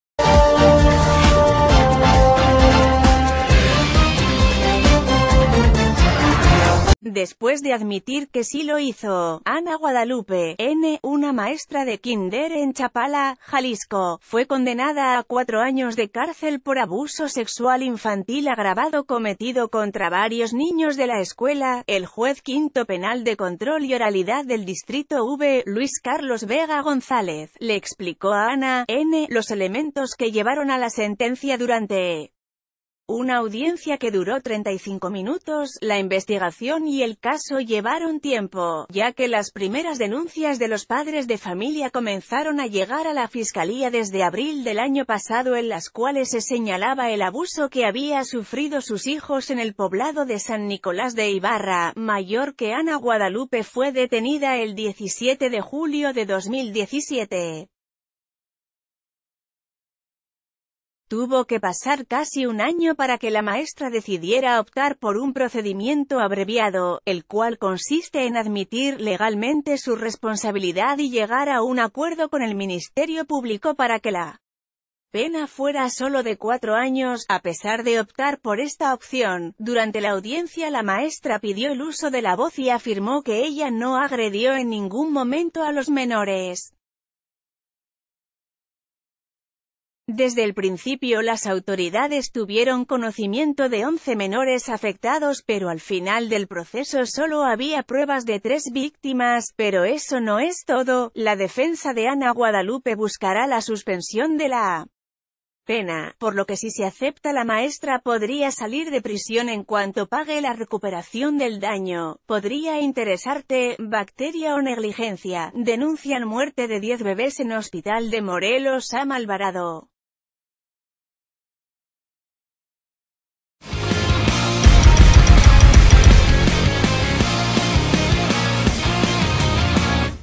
Condenan a maestra de kínder que abusó sexualmente de niños en Jalisco
Después de admitir que sí lo hizo, Ana Guadalupe “N”, una maestra de kínder en Chapala, Jalisco, fue condenada a 4 años de cárcel por abuso sexual infantil agravado cometido contra varios niños de la escuela. El Juez Quinto Penal de Control y Oralidad del Distrito V, Luis Carlos Vega González, le ex...